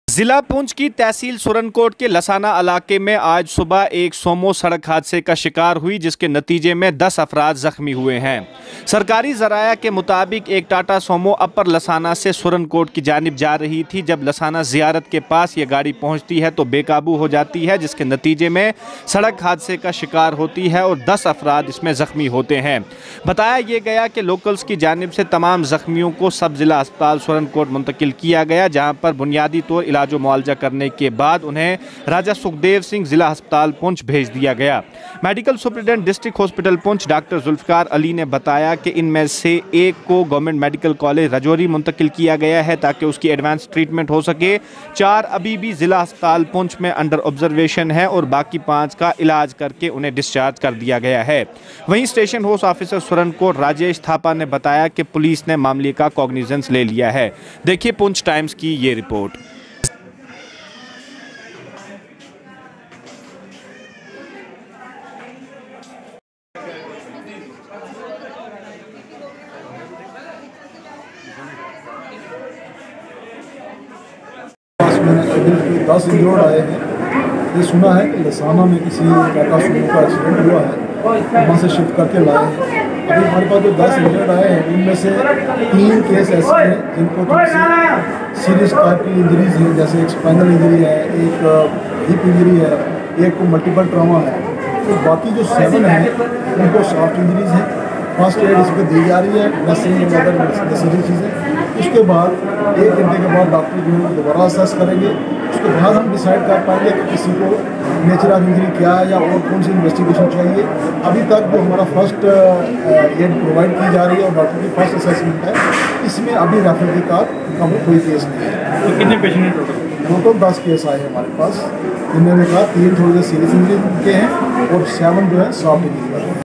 10 inju-red in Sumo acci-dent at Lassana area of Surankote, one shifted to GMC Rajouri.
10 injured in Sumo accident at Lassana area of Surankote, one shifted to GMC Rajouri, 4 under observation in DH Poonch | Waseem Haidery reports.